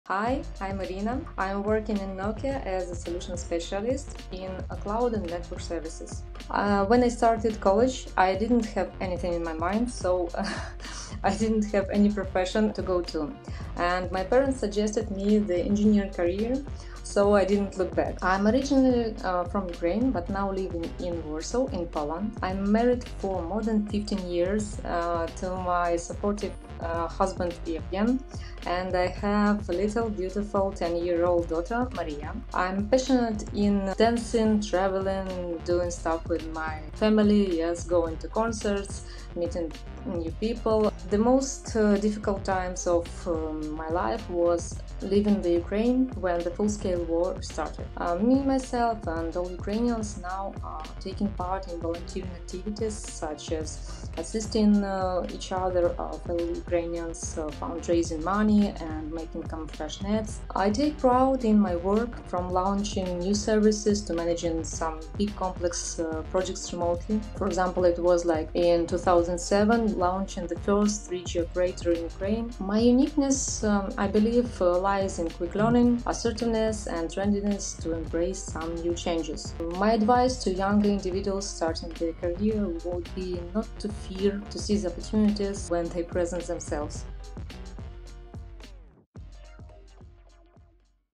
Meet Irina, Solution Specialist | Nokia Poland
Meet Irina, a Solution Specialist from Nokia in Poland.  Hear her talk about her role at Nokia, and how she supports other refugees from her homeland in Ukraine.

To find out more about how you can be part of TeamNokia, check out our open positions